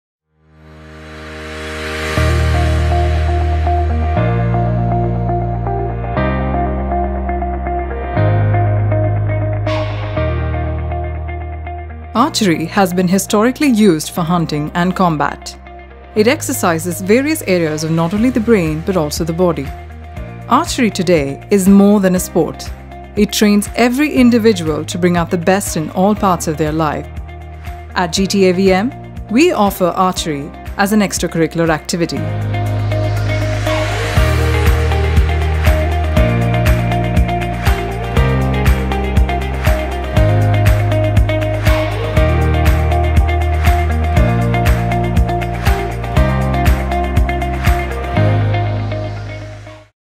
GTGI : Archery in School | GTA Vidhya Mandir CBSE School in Chennai | Neelankarai School | ECR
GTA Vidhya Mandir develop your Archery Skill in CBSE School  in Chennai at Neelankarai (ECR)